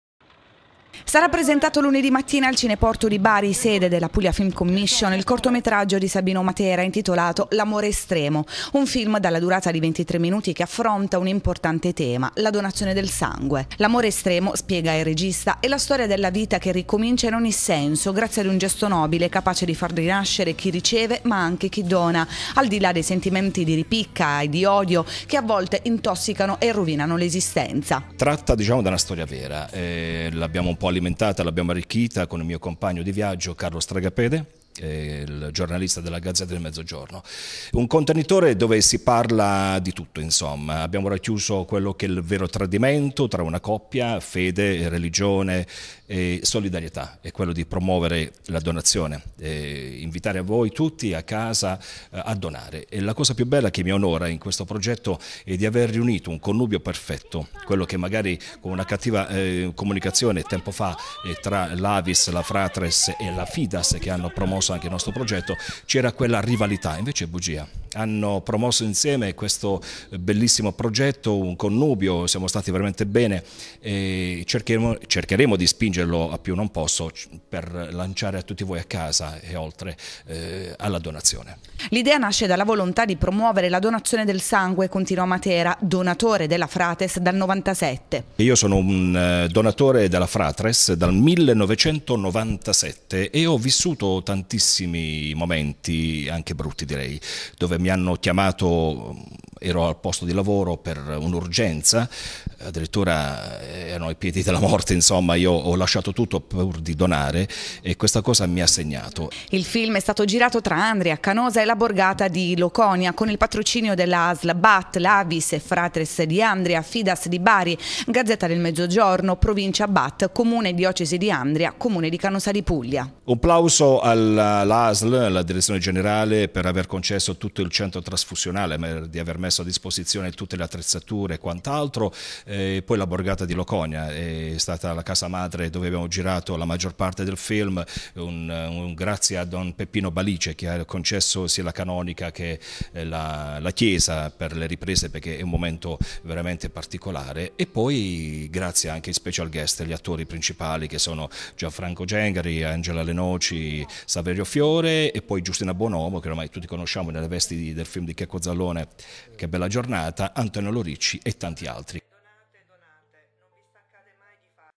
Andria | “L'amore estremo”: il corto di Sabino Matera sarà presentato al Cineporto di Bari
Sarà presentato lunedì mattina al Cineporto di Bari il cortometraggio di Sabino Matera dal titolo “L'amore estremo”. Il film affronta l'importante tema della donazione del sangue.